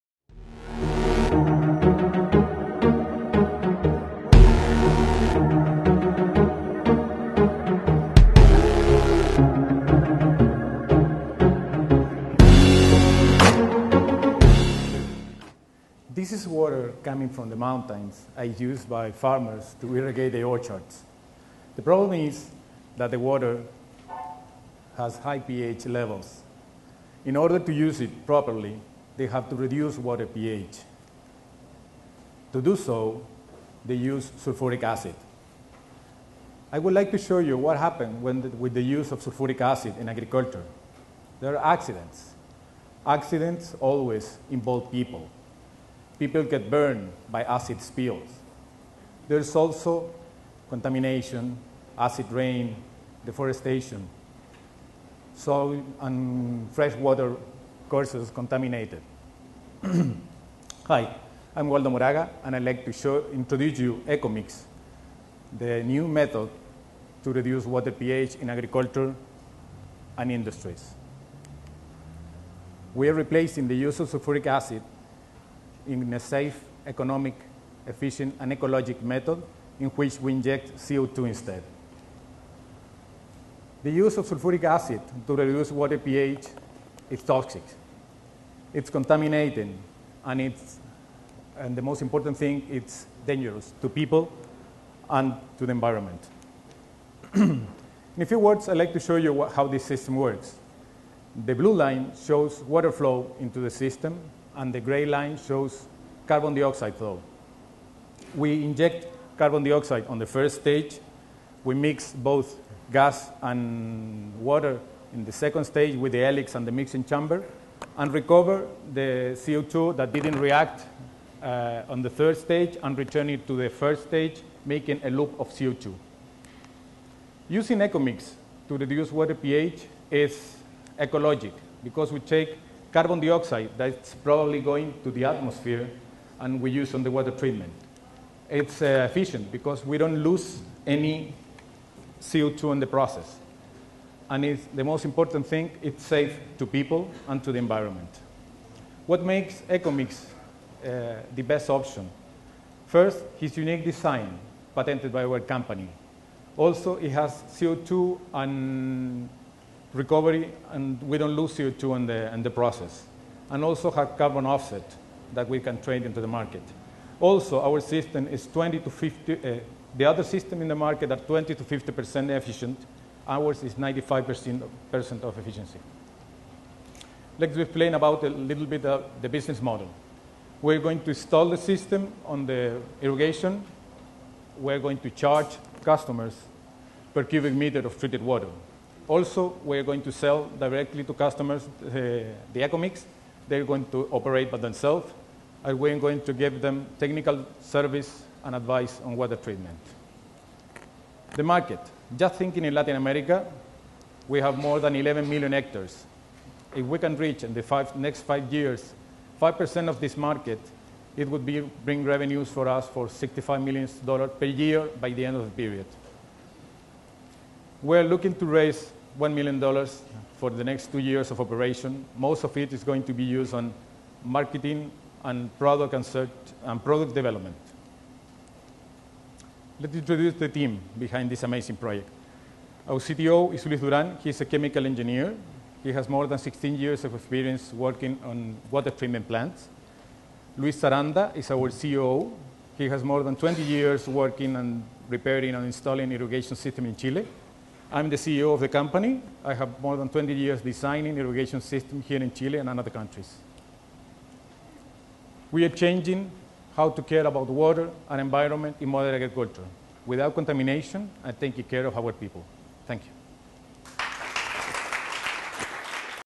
Eco2Mix Pitch Startup Chile Generation 11th Demo Day
We are a group of professionals with a lot of background experience working on irrigation and water treatment systems in agriculture and industry.
Our mission is to design the best ecological solution for water pH control in irrigation systems and other water treatment industry, helping the environment by replacing the use of toxic and dangerous Sulfuric Acid with CO2 instead.